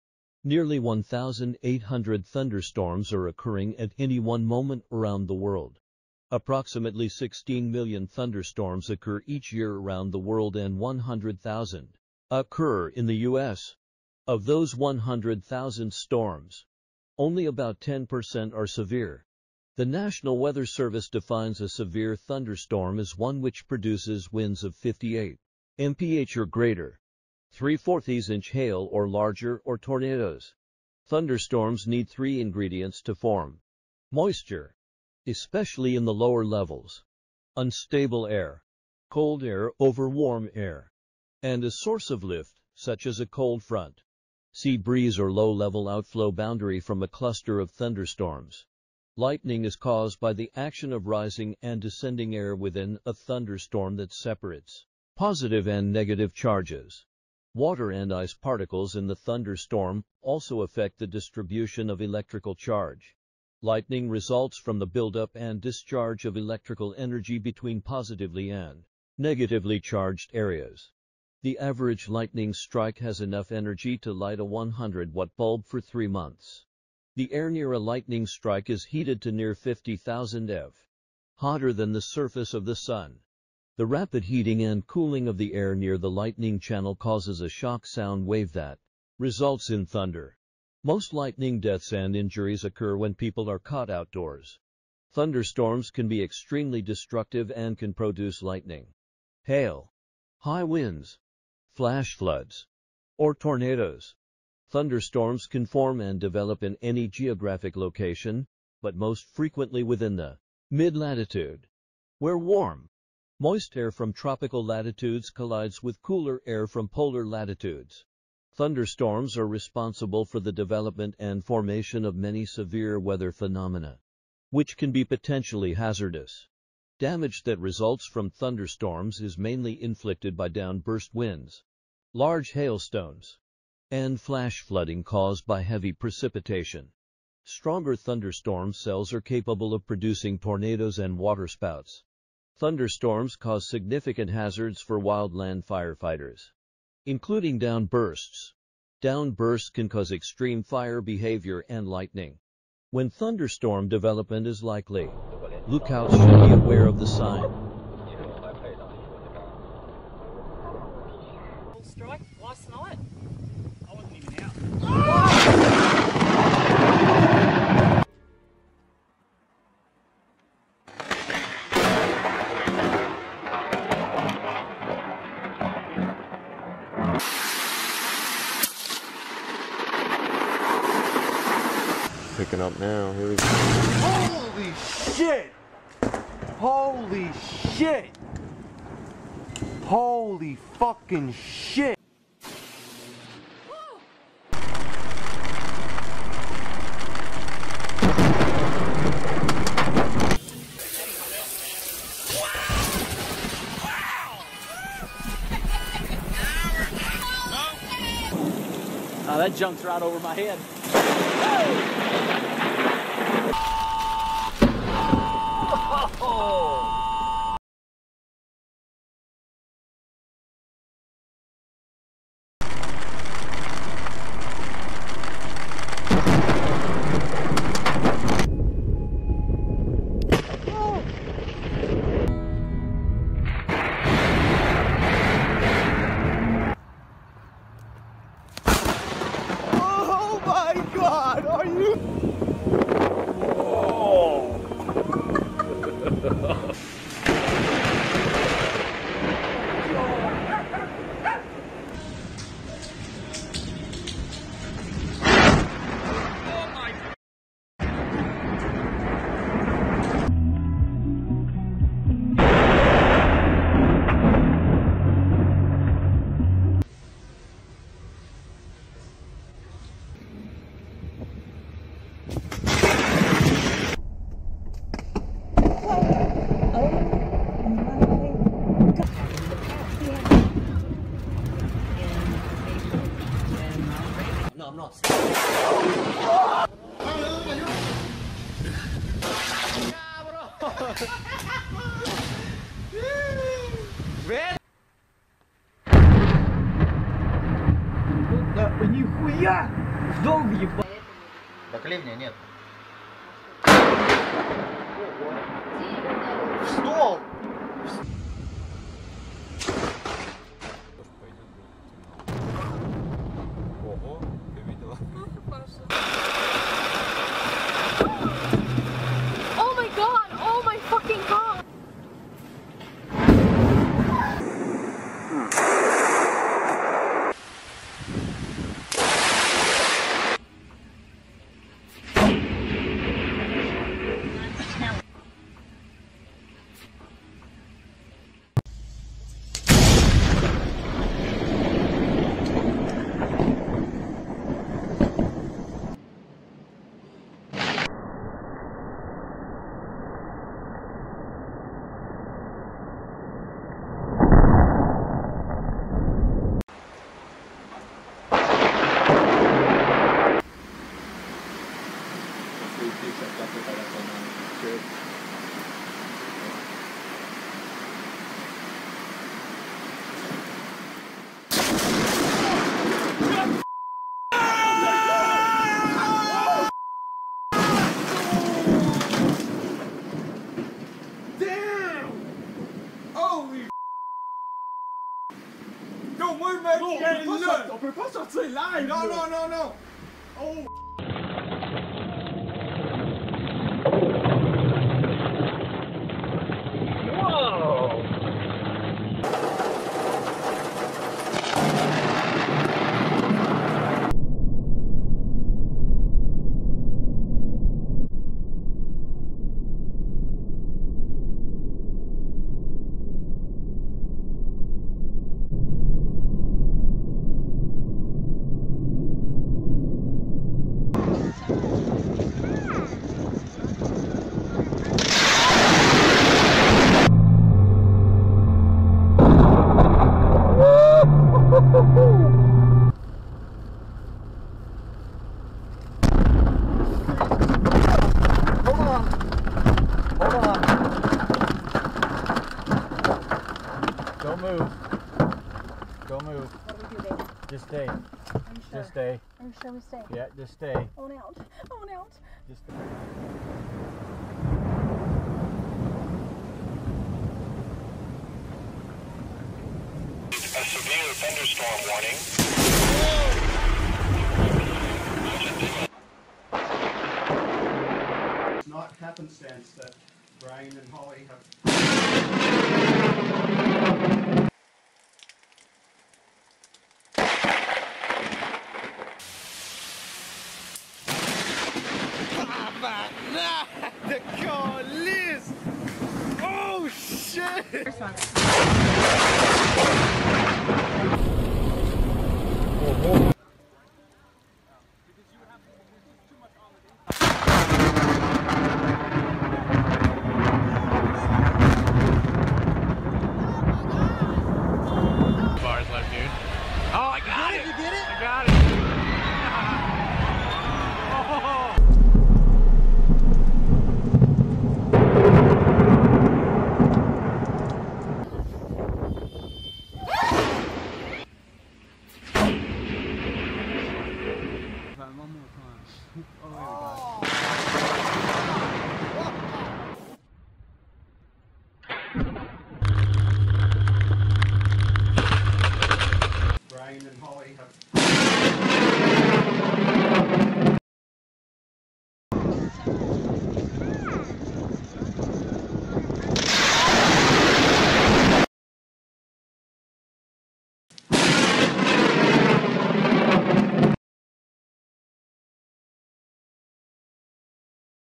Thunderstorm disaster | One of the most mysterious natural disasters
Nearly 1,800 thunderstorms are occurring at any one moment around the world. Approximately 16 million thunderstorms occur each year around the world and 100,000 occur in the U.S. Of those 100,000 storms, only about 10% are severe. The National Weather Service defines a severe thunderstorm as one which produces winds of 58 mph or greater, 3/4 inch hail or larger or tornadoes.

Thunderstorms need 3 ingredients to form; moisture, especially in the lower levels, unstable air, cold air over warm air, and a source of lift such as a cold front, sea breeze or low level outflow boundary from a cluster of thunderstorms.